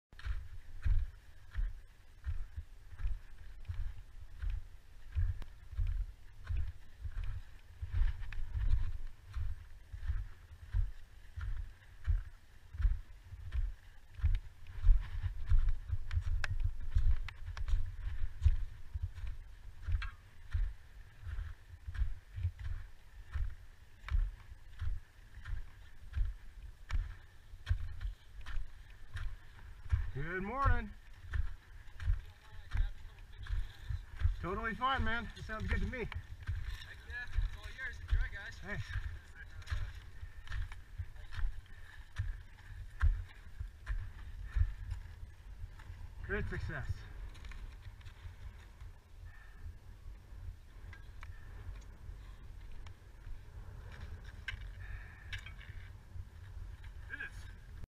Mt Hood Summit Arrival at Sunrise
Arriving at the summit of Mt. Hood, at sunrise, on May 29, 2021. Approximately 5:15am.